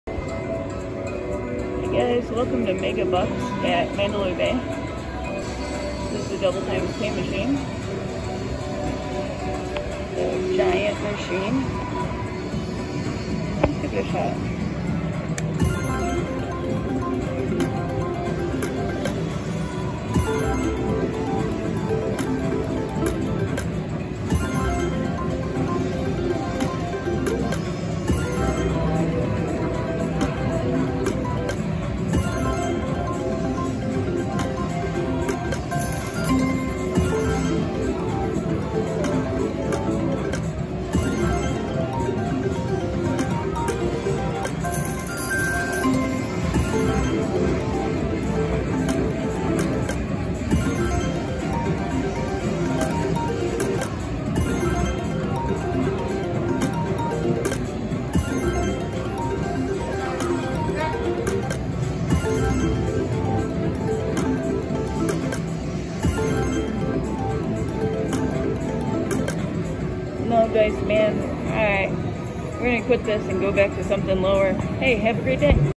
MEGABUCKS 3x4x5x Double Times Pay Slot Machine Live Slot Play at Mandalay Bay Las Vegas Strip!!! 🎉
CasinoCor Gaming ©